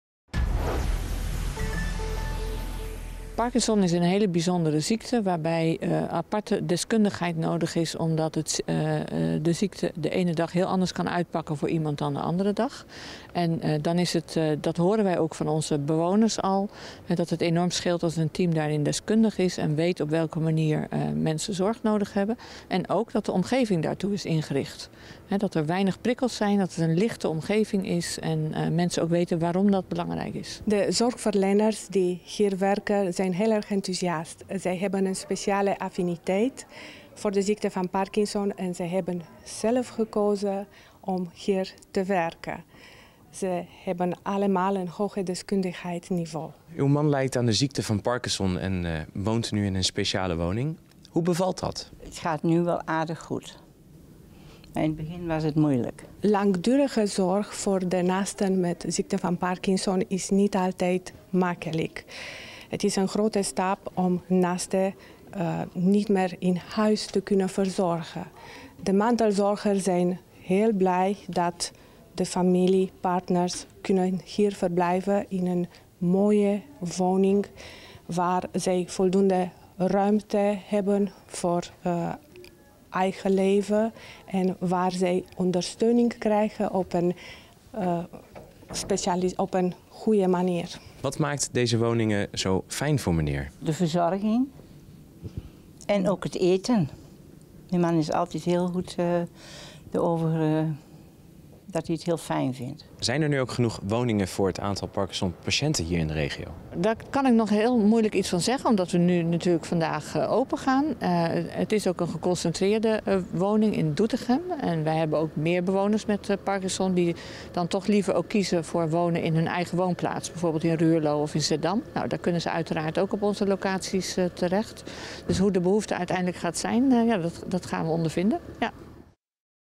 Speciale Parkinsonwoningen geopend in Doetinchem
Speciaal aangepaste woningen zijn woensdag officieel geopend in Doetinchem, voor mensen die lijden aan de ziekte van Parkinson.